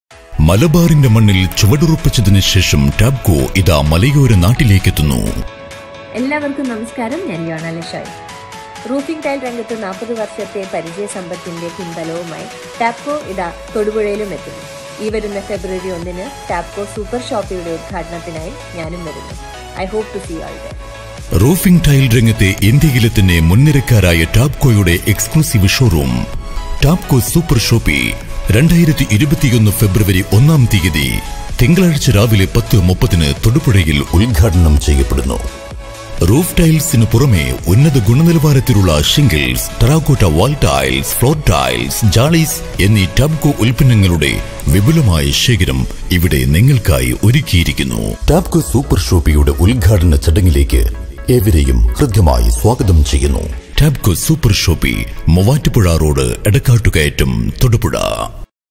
Announcing Tapco super shoppe inauguration at Thodupuzha
TAPCO super Shoppe,  an exclusive showroom for TAPCO,  SHIELDER & AQUAIZOL products is getting inaugurated @ Thodupuzha on 1st February,  2021